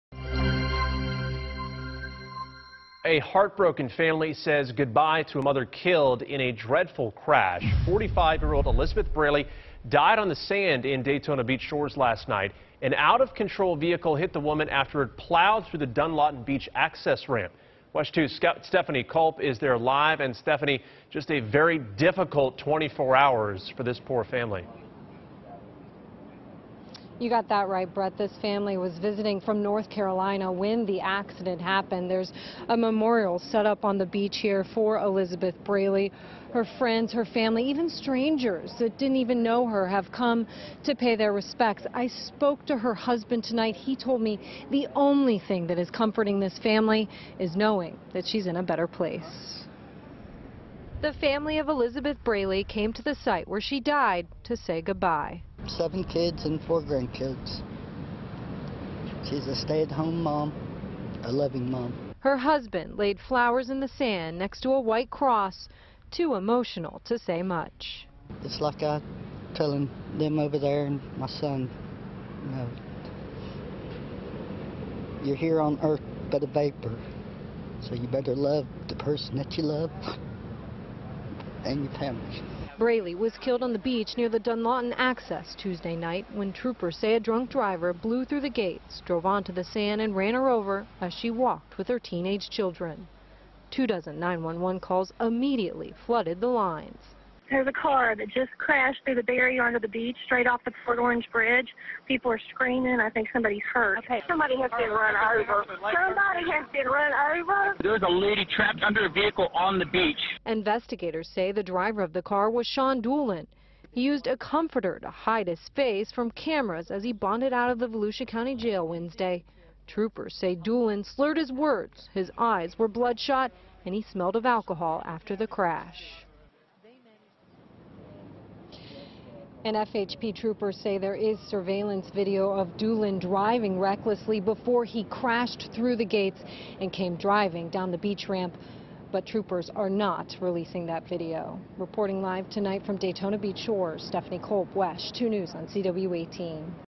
Vigil held for woman fatally struck at beach
The family of a woman who was struck by a vehicle and killed on a Volusia County beach holds a vigil in her memory.